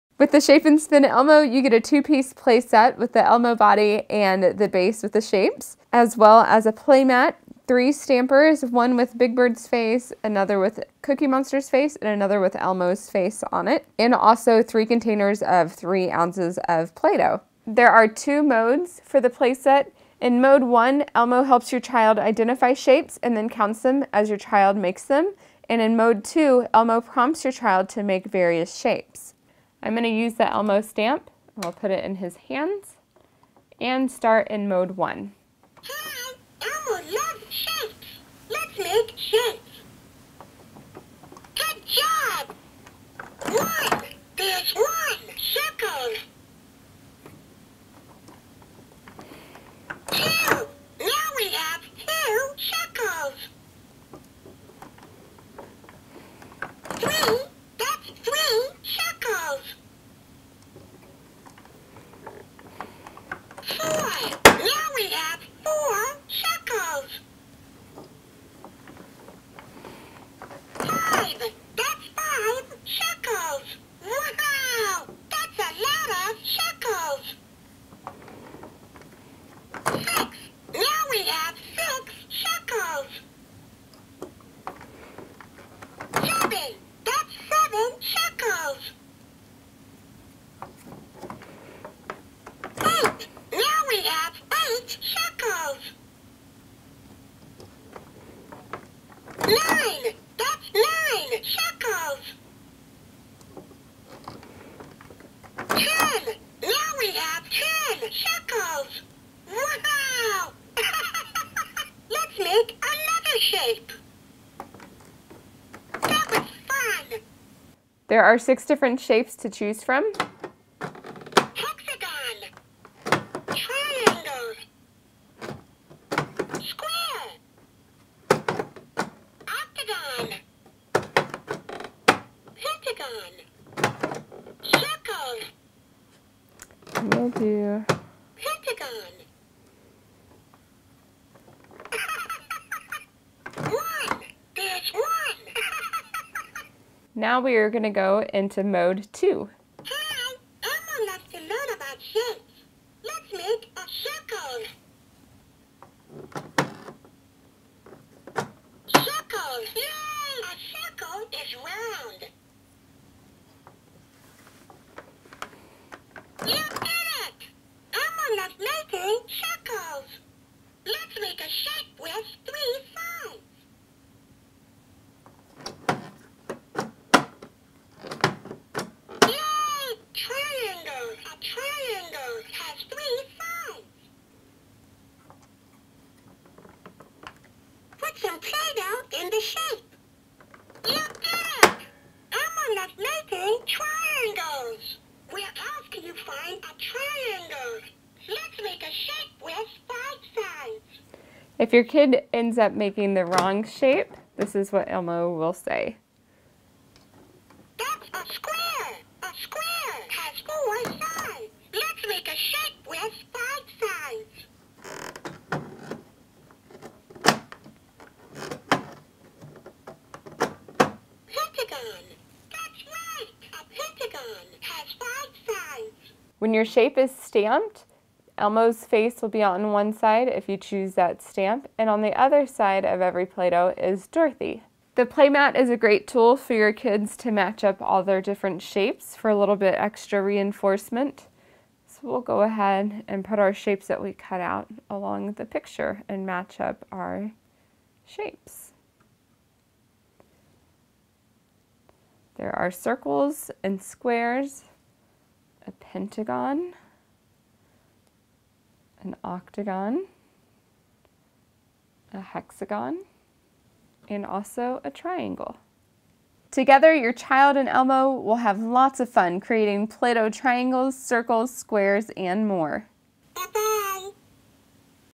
Elmo Teaches Shapes with Play-Doh
Using the Elmo Shape 'n Spin Play-Doh Playset is a great tool to teach children shapes, including a circle, square, triangle, pentagon, hexagon, and octagon. It's interactive, creative, and fun. Kids love to play with Play-Doh and this helps them learn shapes while they do it.